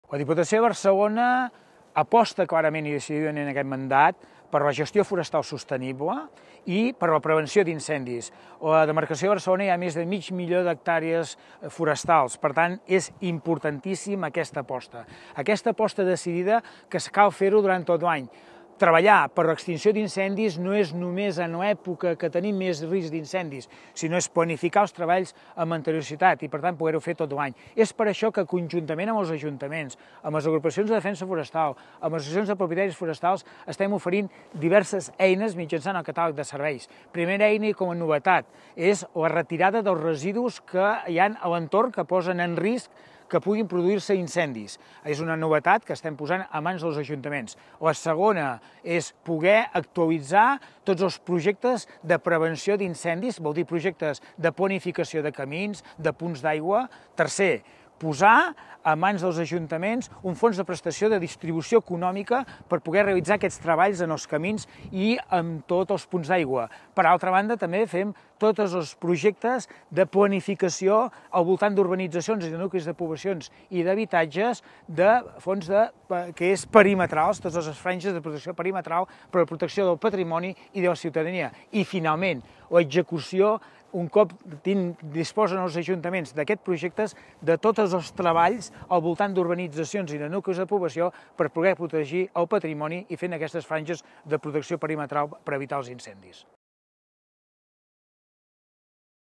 Prevenció d’Incendis i Gestió Forestal
Des de la Diputació de Barcelona apostem per la gestió forestal sostenible i la prevenció d’incendis. Per això, treballem amb els municipis, les ADF i les associacions de propietaris forestals per protegir les més de 500.000 hectàrees forestals de la província de Barcelona.

El diputat de Prevenció d’Incendis i Gestió Forestal, Jordi Fàbrega, explica alguns dels recursos més destacats, com la retirada de residus que puguin afavorir la propagació de les flames, l’actualització dels plans de prevenció d’incendis, una prestació econòmica perquè els municipis puguin habilitar punts d’aigua en camins, o l’elaboració de franges per a la protecció del patrimoni i de la ciutadania.